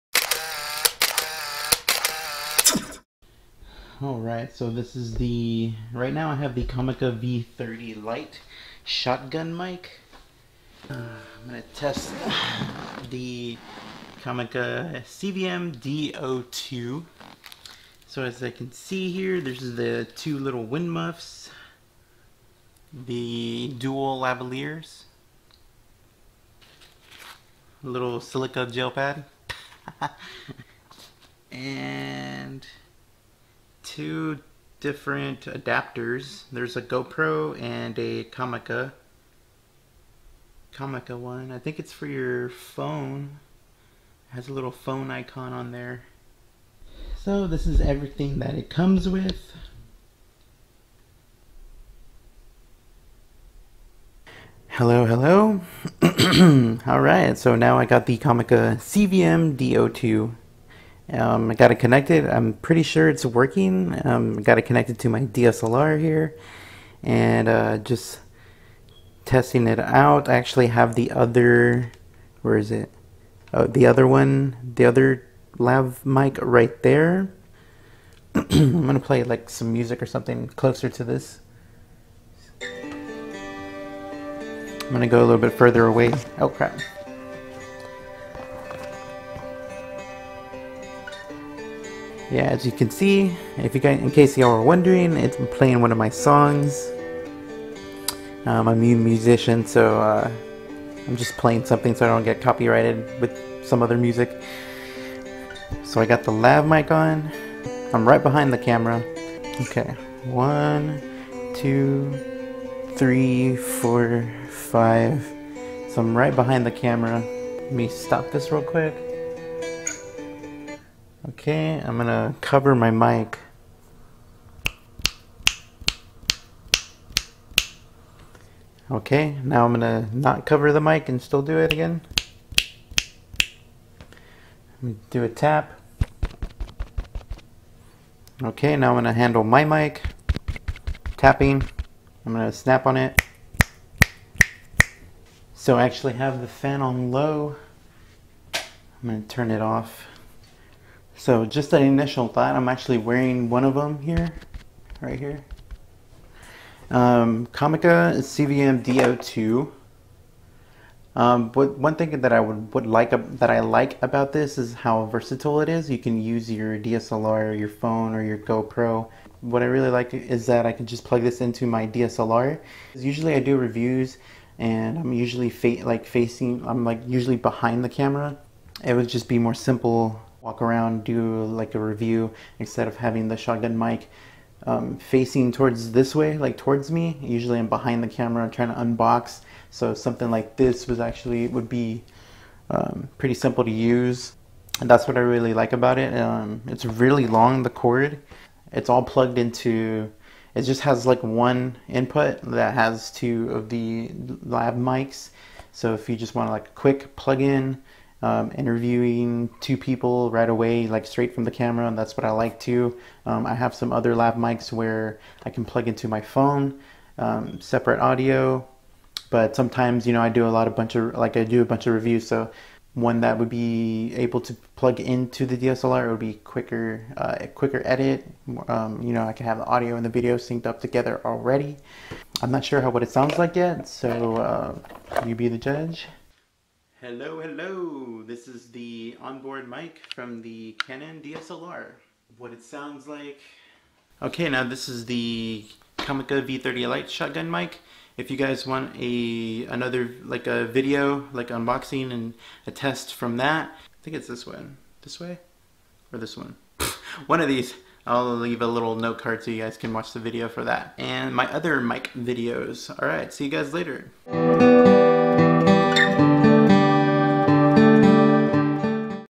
Budget Dual Lav Mic🎤 | Comica CVM D02
I just heard this comica cvm d02 with my headphones and it had such clarity and very little hiss compared to my comica v30 lite shotgun mic. All audio is RAW UNEDITED!
Came with 2 windmuffs and 2 adapters for go pro or smartphone!
You can use this on your DSLR, GOPRO or SMARTPHONE.